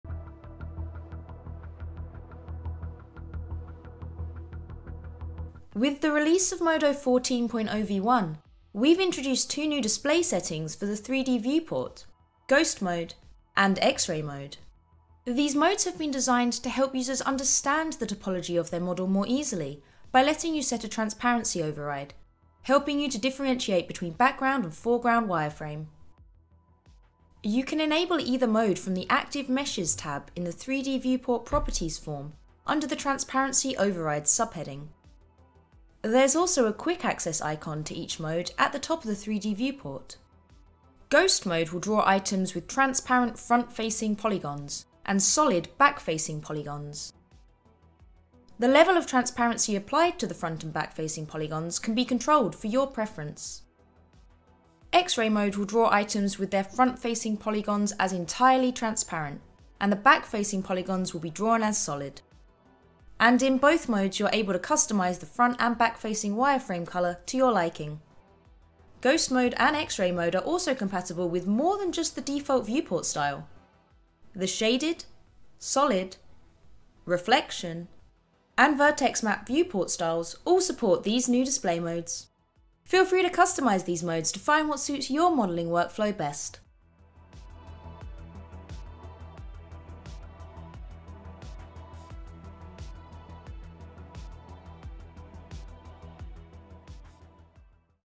Modo 14.2 - Ghost and Xray Modes
Two new display settings have been added for the 3D viewport in Modo 14.0v1: Ghost Mode and Xray Mode.